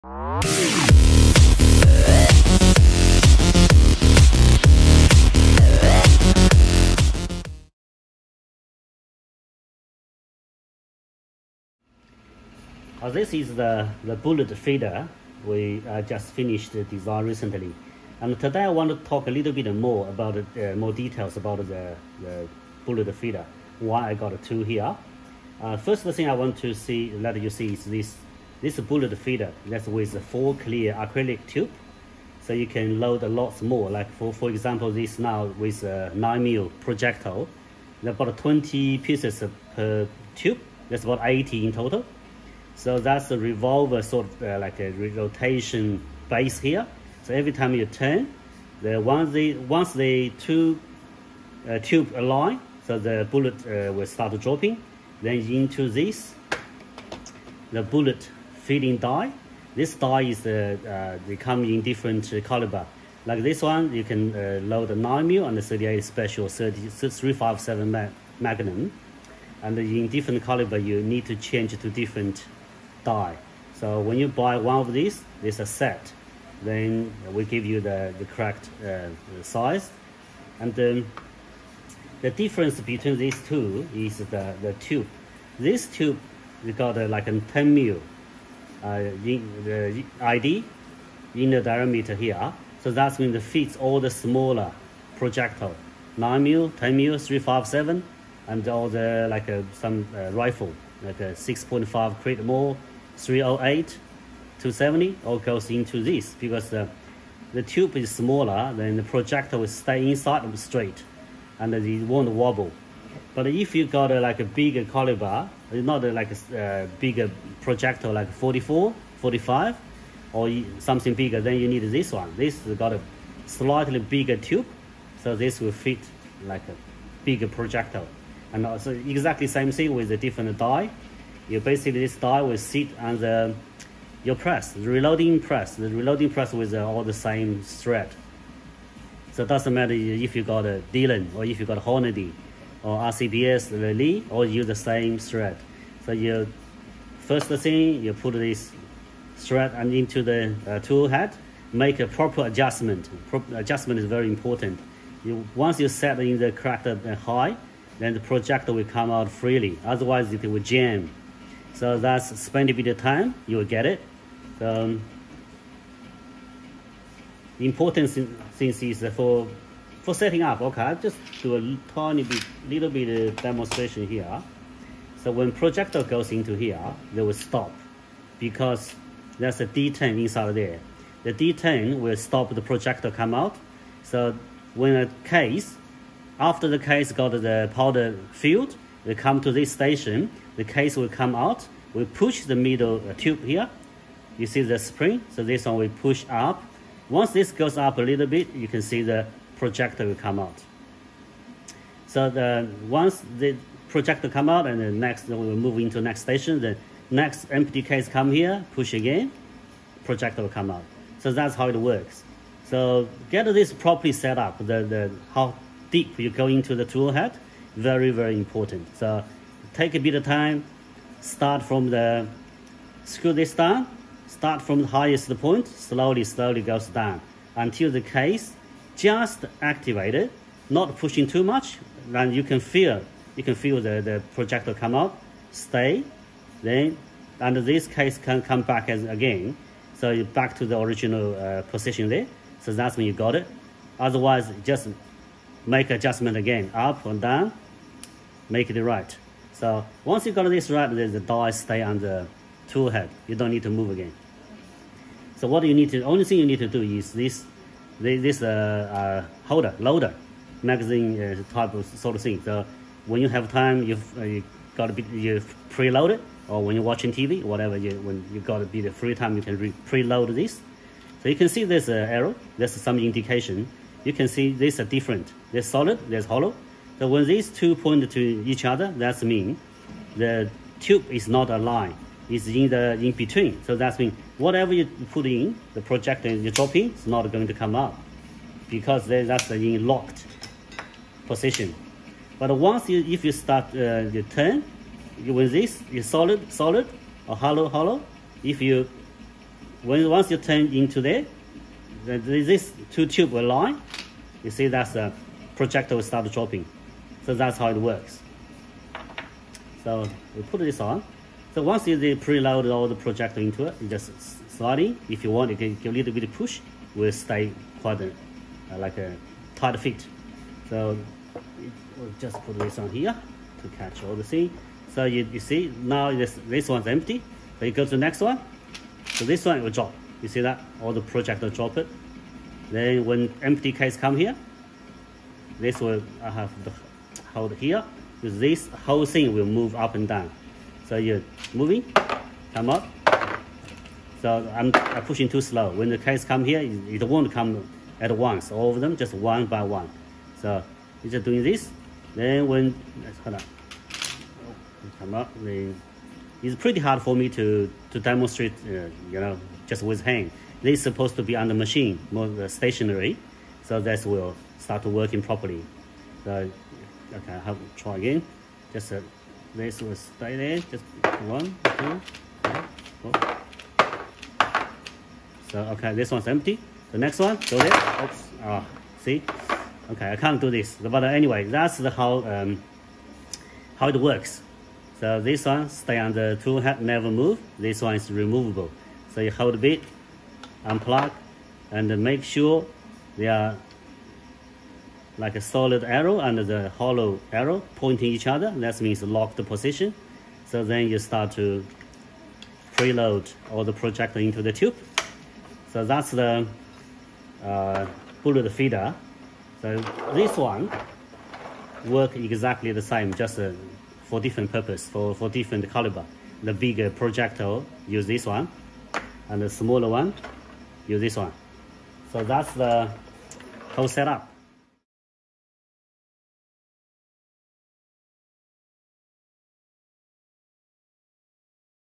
Rotation multi-tube Bullet feeder explained
More details about rotation multi tube bullet feeder made by JC custom.
Now in two sizes to suit bigger or smaller projectile.
Every set is included the bullet feeding die ( caliber specified ) and the rotation multi tube bullet holder.